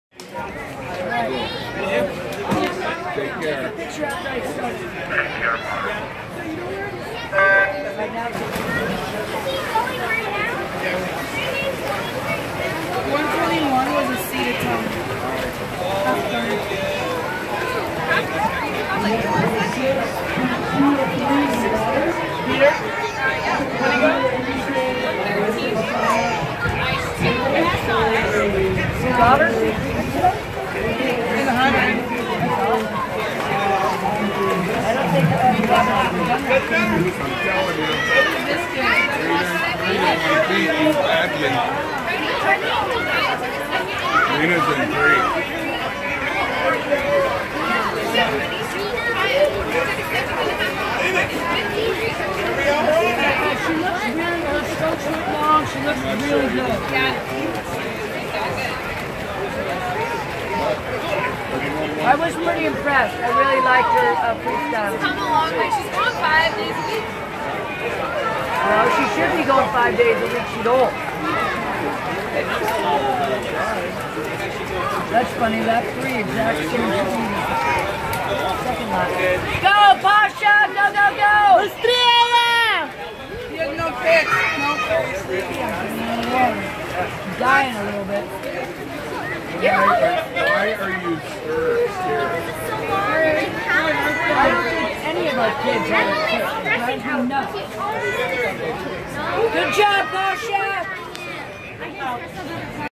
Patrick swims 100 Free
Championship 10&under in Sunnyvale 2018